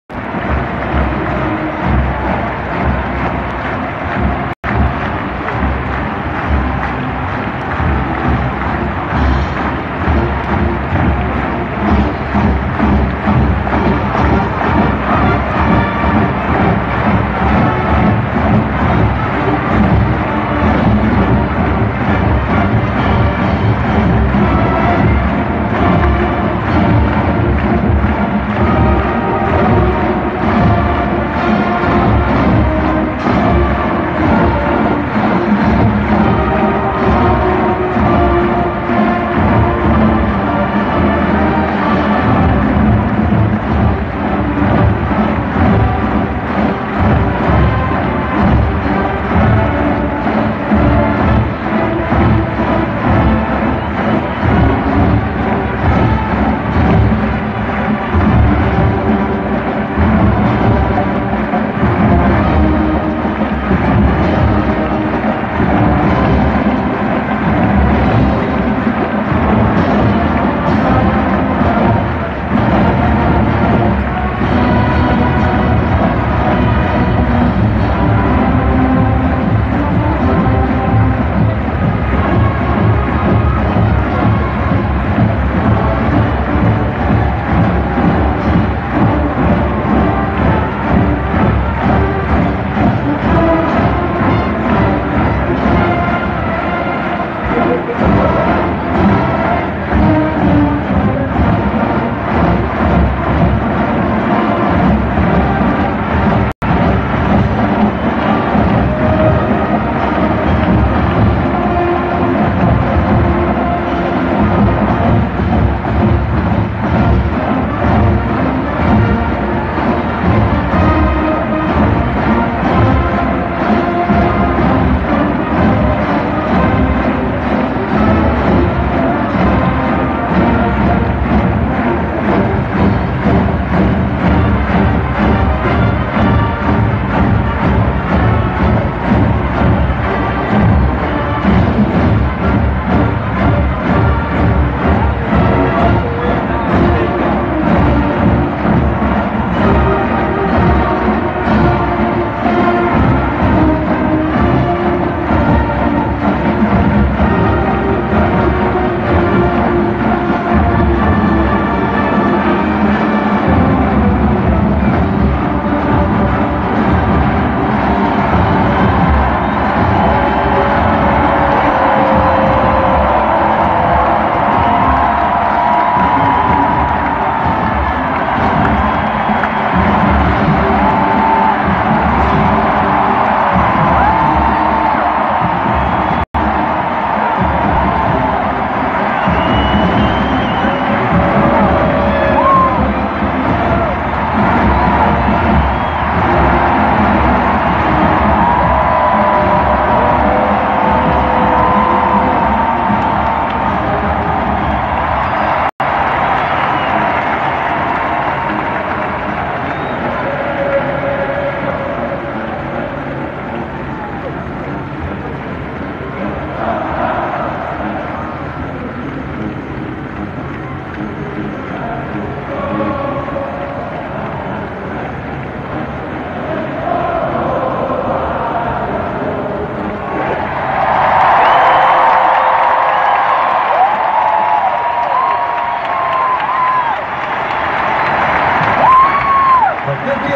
Quad Script Ohio 360 9-22-2018
Quad Script Ohio during the OSU -Tulane game on alumni band day. Featuring Anthony Violi (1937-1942), a 100 year-old trumpet player as special guest. Shot from row 45 (top of the scoreboard row in handicap seating) South stands.